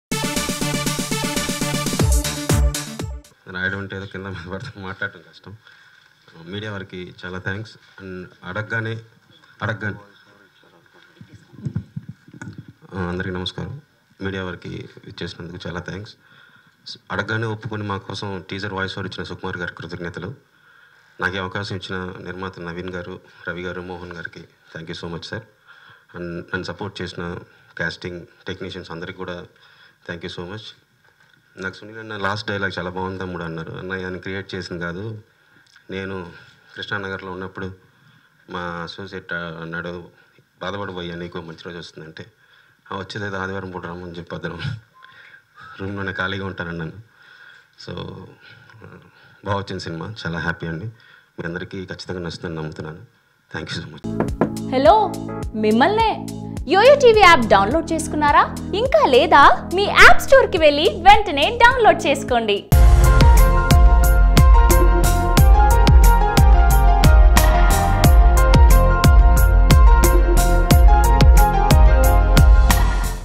Kishore Tirumala Speech At Chitralahari Teaser Launch | Sai Dharam Tej | YOYO Cine Talkies
Kishore Tirumala Speech At Chitralahari Teaser Launch